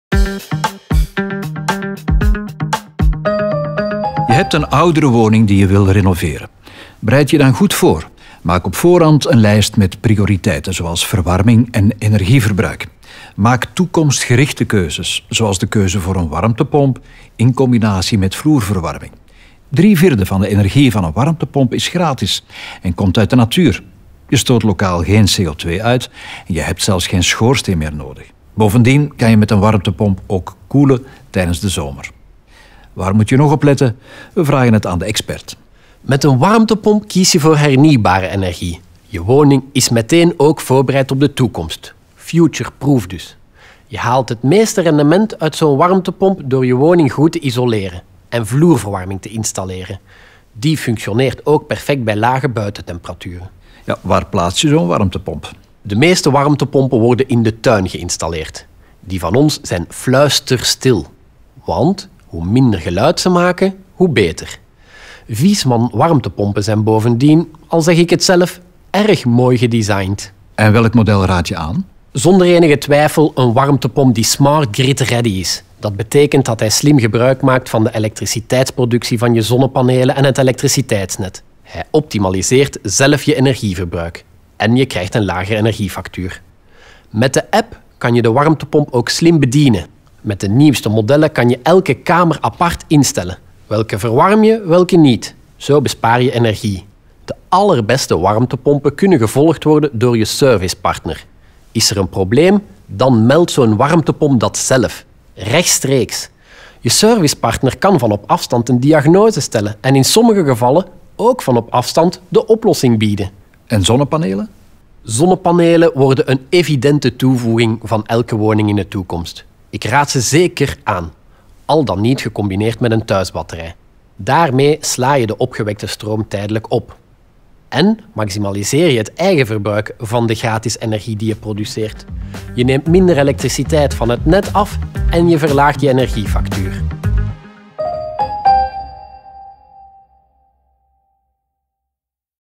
Een warmtepomp kiezen: waar moet je op letten?
Ga je renoveren? Dan is het belangrijk om toekomstgerichte keuzes te maken op het gebied van verwarming en energiegebruik. Een warmtepomp in combinatie met vloerverwarming is zo’n future proof keuze. Drievierde van de energie van een warmtepomp komt uit de natuur en is dus gratis. Daarnaast stoot je lokaal geen CO2 uit en heb je geen schoorsteen meer nodig. Waar moet je op letten bij de keuze voor een warmtepomp? We vragen het aan de expert